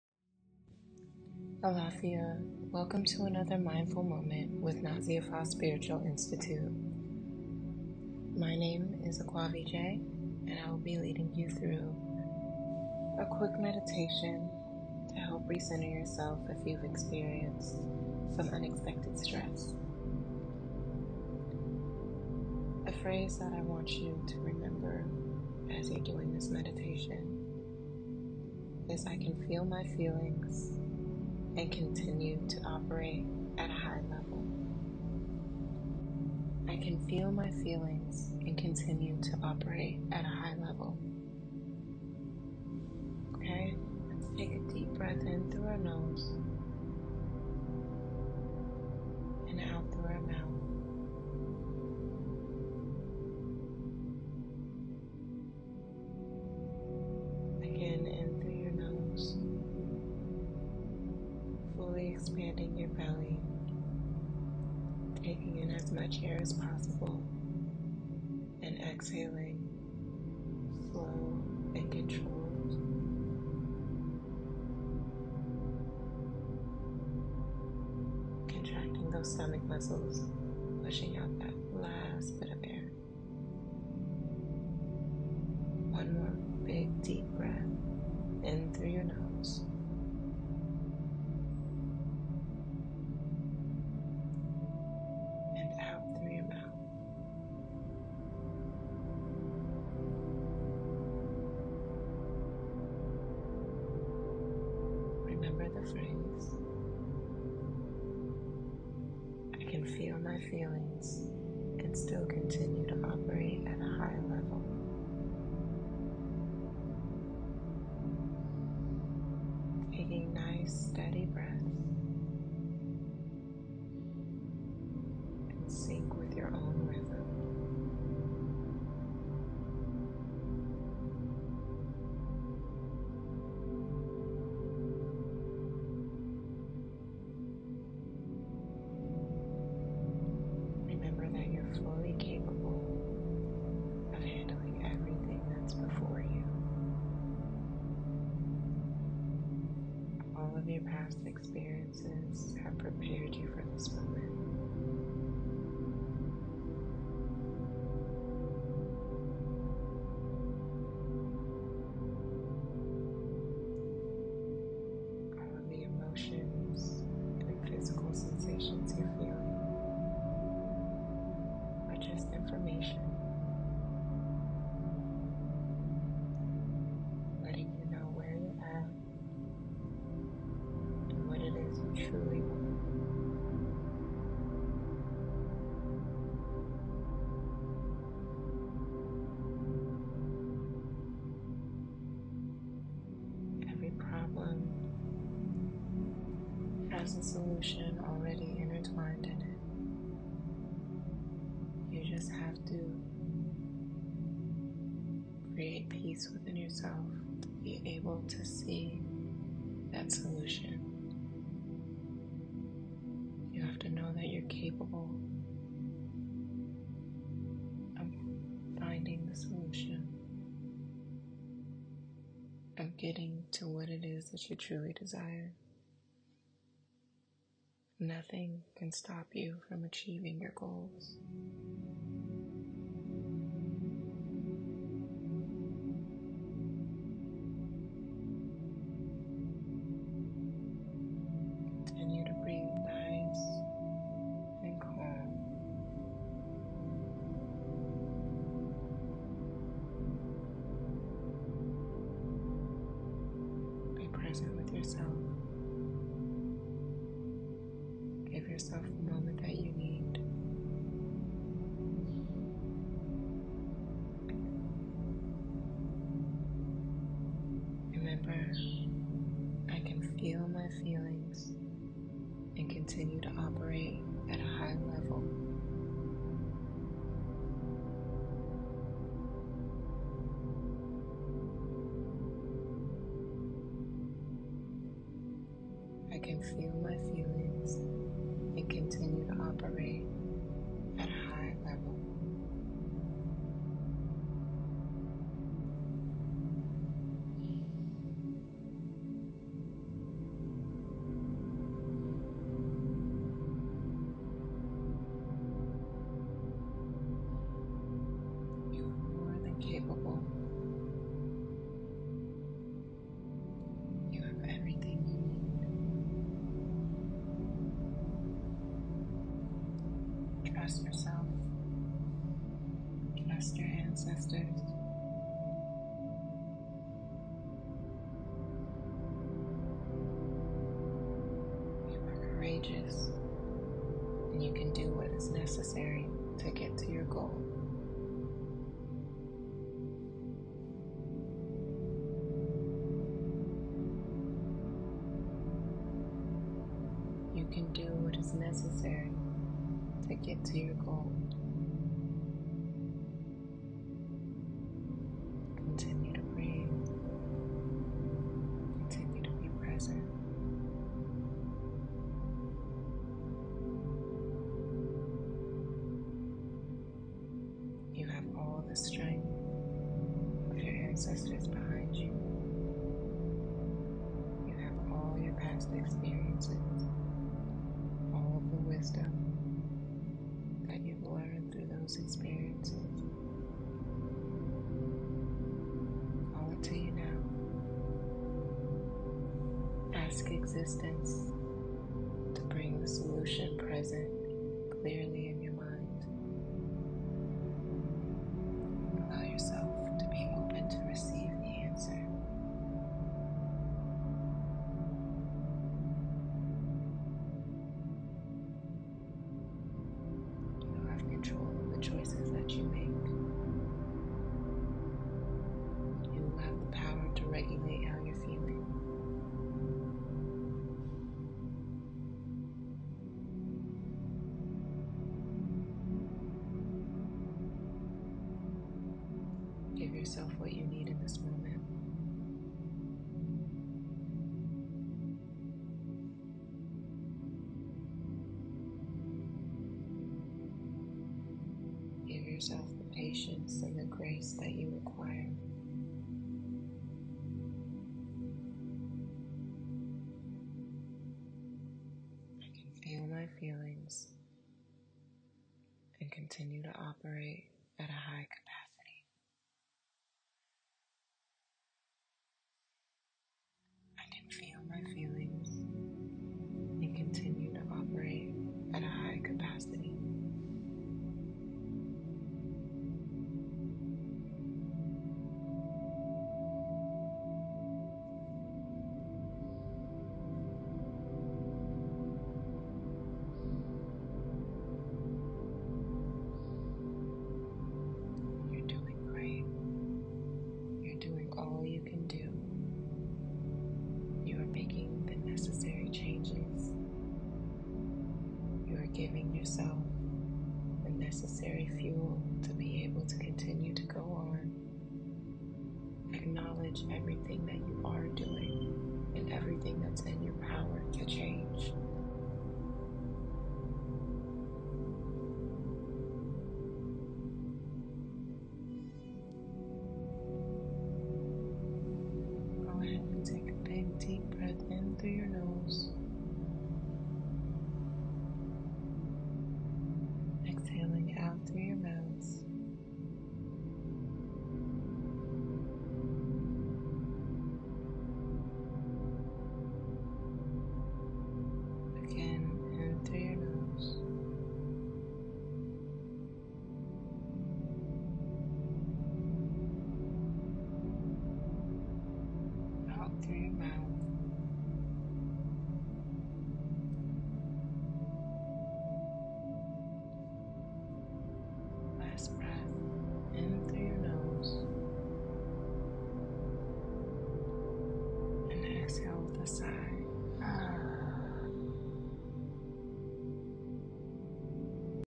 Unwind and De-stress with Aquavee J's 10 Minute Meditation Routine | Relaxation Hack
Escape the chaos of everyday life and join Aquavee J in a soothing meditation routine designed to help you unwind and de-stress. This relaxation hack is perfect for those looking to find inner peace and calmness amidst the hustle and bustle.

Let the gentle guidance of Aquavee J lead you to a state of tranquility and mindfulness. Say goodbye to stress and hello to a sense of serenity with this meditation session. Press play and embark on a journey towards a more peaceful mind and body.

Relax, breathe, and let go of all tension with Aquavee J's meditation for stress relief.